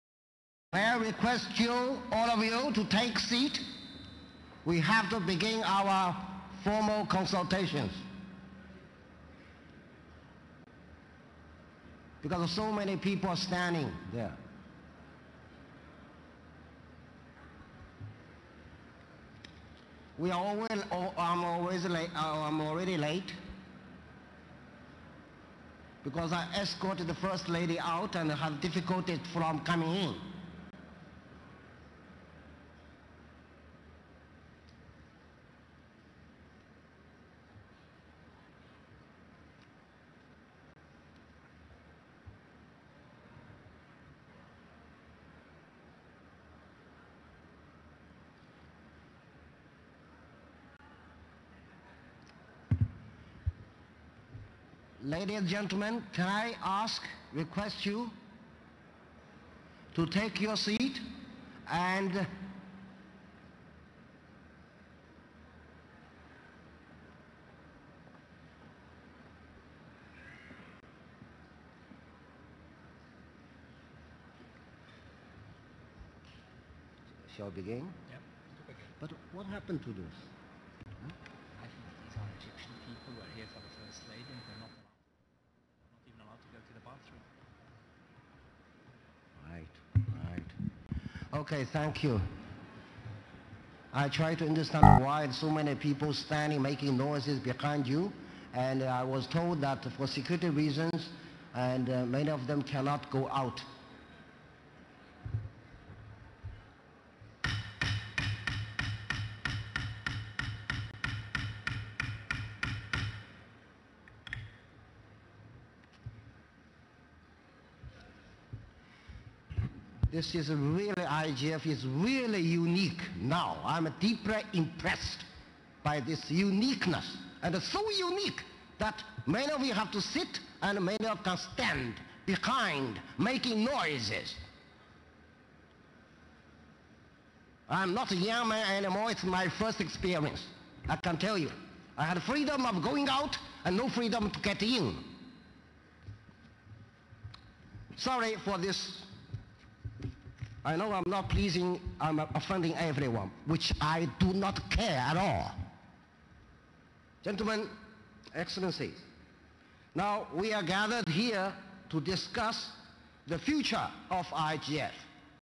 un-sha-loses-it.mov
Sha Zukang, the United Nations Under-Secretary-General for Economic and Social Affairs, loses it on the last day of the Internet Governance Forum in Sharm El Sheikh.

The session was due to start but the room had been locked-down while Egypt's First Lady wandered around outside in the lobby. This meant that lots of people were waiting at the back of the room by the doors rather than taking their seats.

Sha is informed about this but is so wound up that "he does not care". The incident was the talk of the conference.